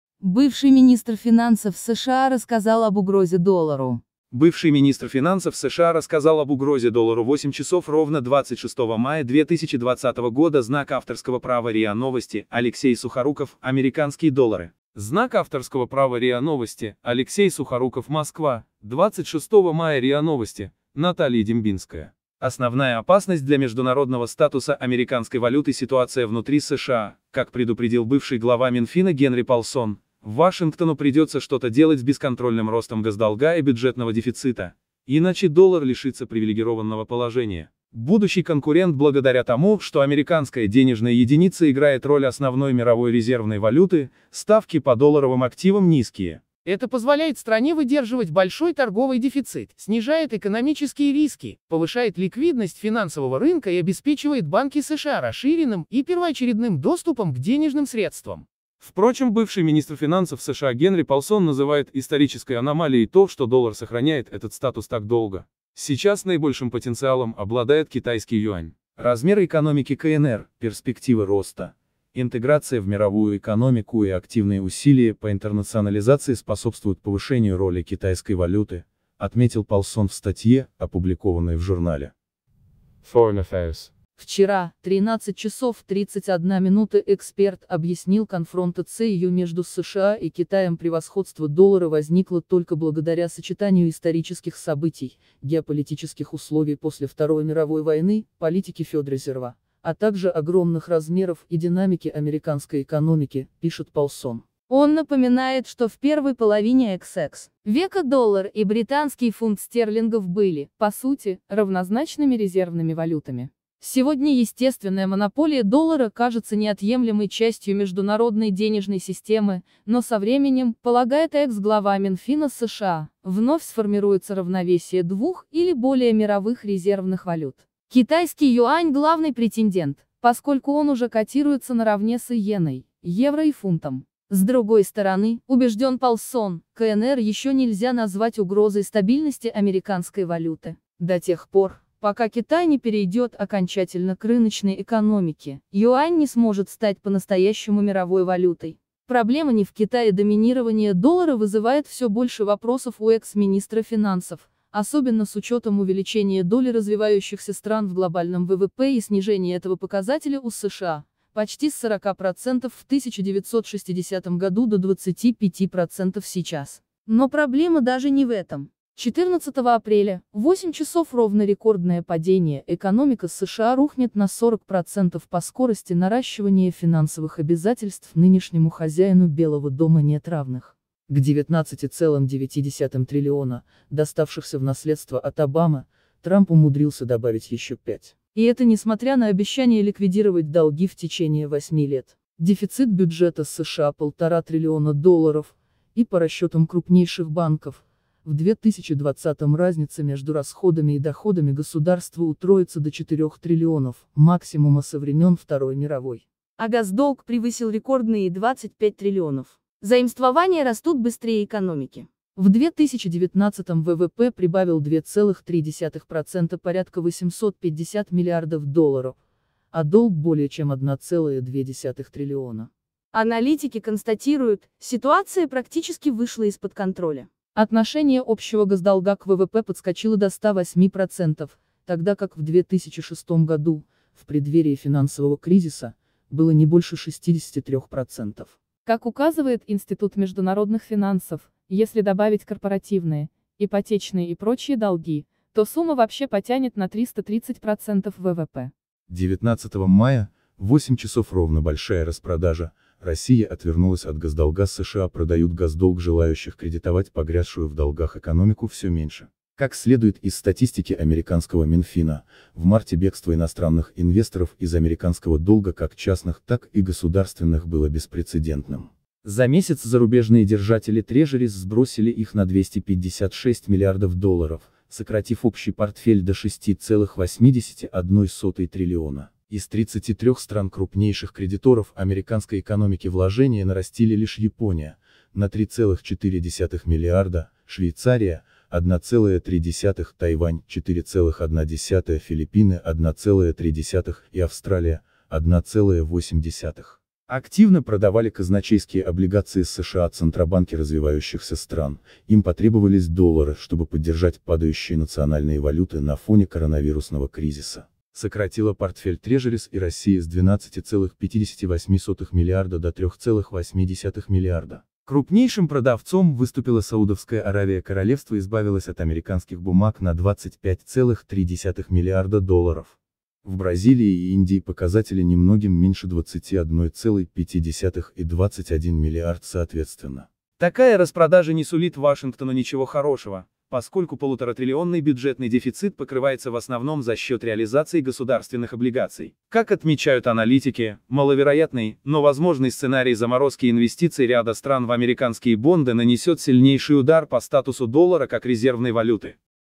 Бывший министр финансов США рассказал об угрозе доллару
Бывший министр финансов США рассказал об угрозе доллару Бывший министр финансов США рассказал об угрозе доллару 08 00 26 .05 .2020 РИА Новости Алексей СухоруковАмериканские доллары РИА Новости Алексей Сухоруков МОСКВА, 26 мая РИА Новости, Наталья Дембинская . Основная опасность для международного статуса американской валюты ситуация внутри США . Как предупредил бывший глава Минфина Генри Полсон, Вашингтону придется что-то делать с бесконтрольным ростом госдолга и бюджетного дефицита, инач